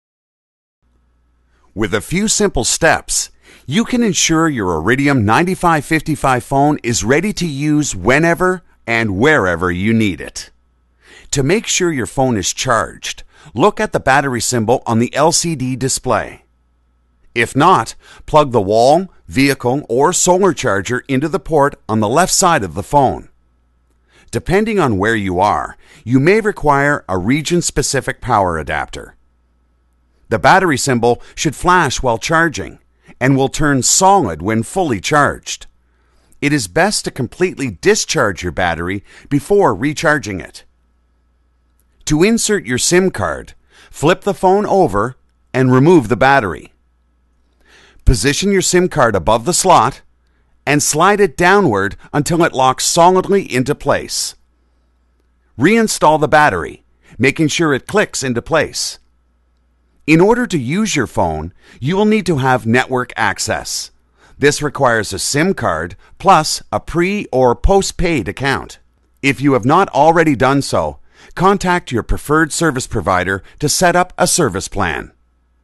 Iridium 9555 Tutorial: Preparing Your Satellite Phone for Use
Make sure your Iridium 9555 is ready to use, wherever and whenever you need it.  Find out how.